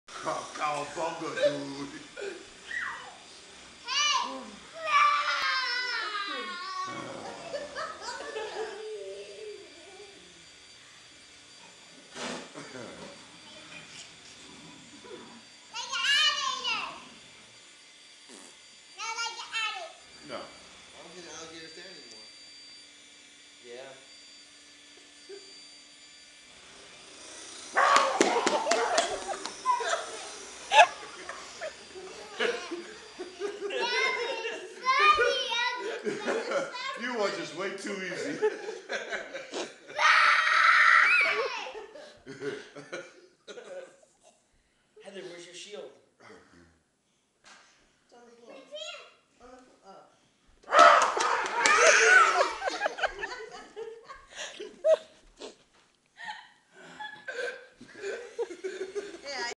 Branchport
A little video with the family while we were in branchport, NY